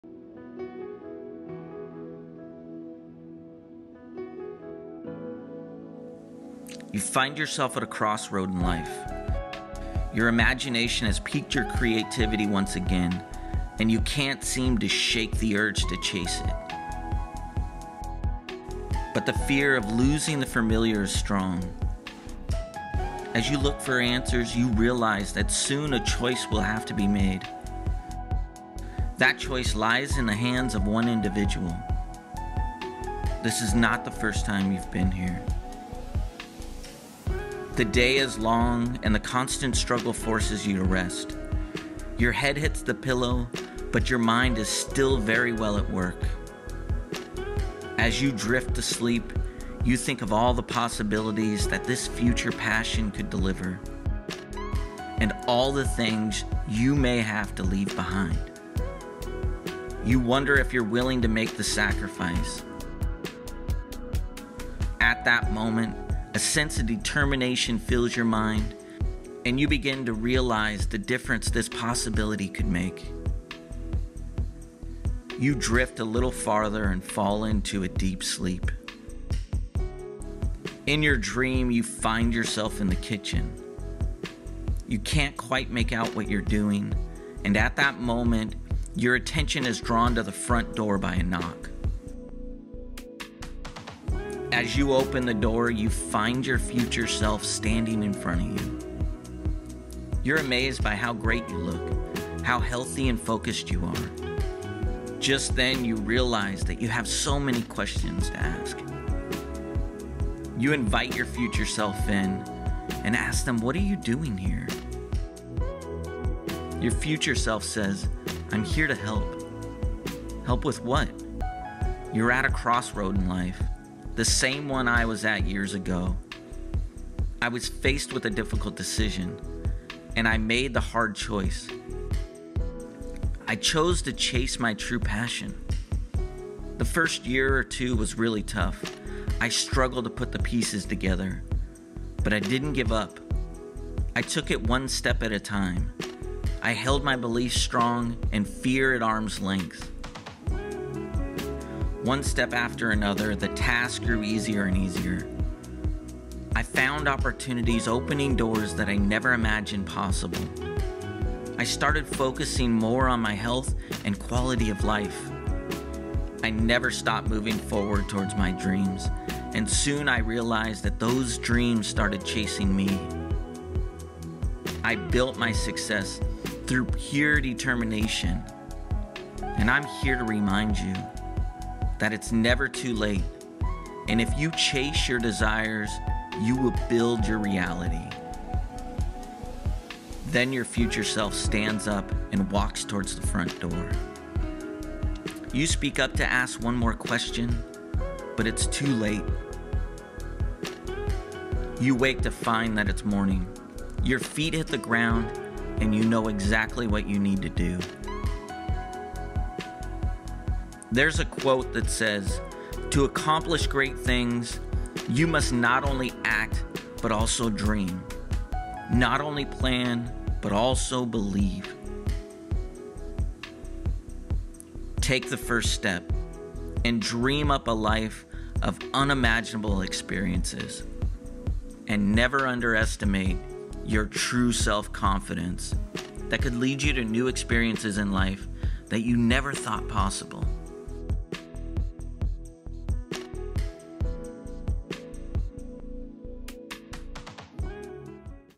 Self Confidence to Chase Your Passion - Inspirational Video
This story will open the mind for possibilities of change. It will help you understand the importance of taking a chance. Shed the fear and develop the courage and resilience to keep move forward.

Speaker:
Mike McAulay

Be a Dreamer does not own the rights to these video clips.